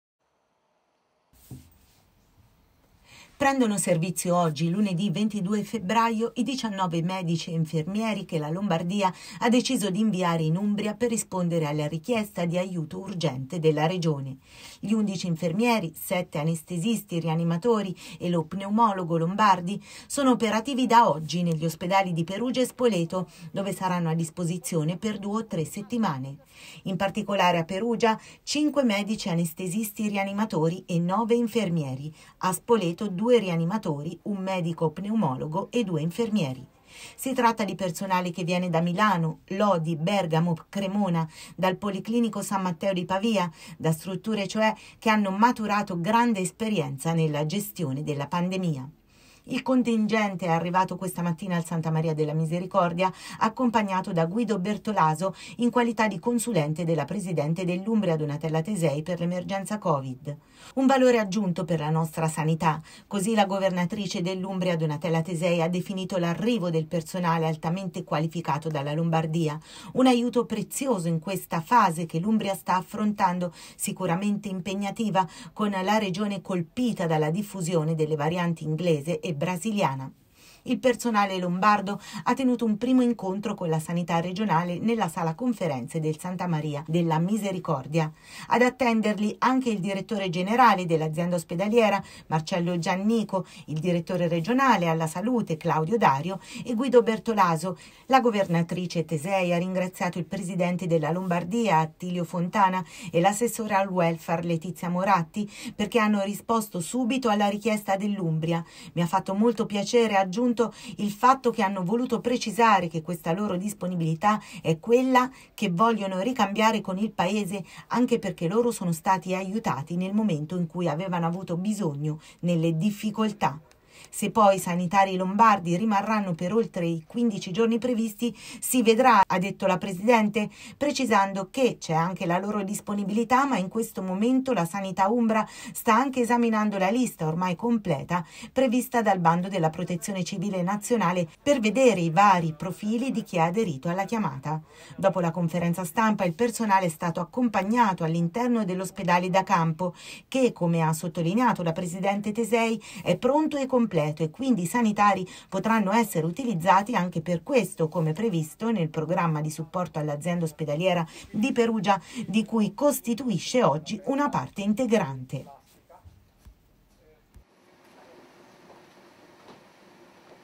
Covid in Umbria: Tesei ringrazia il personale sanitario arrivato dalla Lombardia
La Presidente della Regione Umbria, Donatella Tesei, ha salutato questa mattina il personale sanitario proveniente dalla Lombardia che sarà di supporto all’Ospedale di Perugia per le prossime settimane.

    “Vi ringrazio per la vostra disponibilità – ha affermato – sono sicura che sarete di grande aiuto e spero che a livello umano e professionale vi sarà uno scambio reciproco con tutto il nostro personale che sta svolgendo con grande dedizione la propria attività”.

    Insieme alla Presidente, c’era il consulente Covid della stessa Presidente, Guido Bertolaso, che ha a sua volta ha accolto e ringraziato “gli operatori per il loro spirito e la loro disponibilità alla collaborazione in questo momento di emergenza”, così come hanno fatto il direttore della Sanità umbra, Claudio Dario, che ieri sera aveva già ricevuto insieme al Direttore Stefano Nodessi il personale sanitario al suo arrivo in Umbria, e il direttore generale dell’Ospedale di Perugia Marcello Giannico.

   Al briefing hanno partecipato tra gli altri anche i direttori dei reparti Anestesia e Rianimazione 1 e 2, Gori e De Robertis e il Direttore Sanitario Bianchi. Gli operatori presenti questa mattina a Perugia fanno parte del più ampio contingente, con notevole esperienza sul campo, che sarà operativo nello stesso capoluogo e a Spoleto, composto in tutto da 19 sanitari, di cui 7 medici anestesisti/rianimatori e 1 pneumologo, e 11 infermieri delle Asst lombarde.

   Alla fine dell’incontro la presidente Tesei, Bertolaso, Dario e Giannico si sono recati all’ospedale da campo, dove è stato sottolineato che la struttura è pronta e completa e che i sanitari potranno se necessario essere utilizzati anche per questo “reparto distaccato” come previsto nel programma di supporto all’Azienda ospedaliera di Perugia di cui l’ospedale da campo ne è attualmente parte integrante.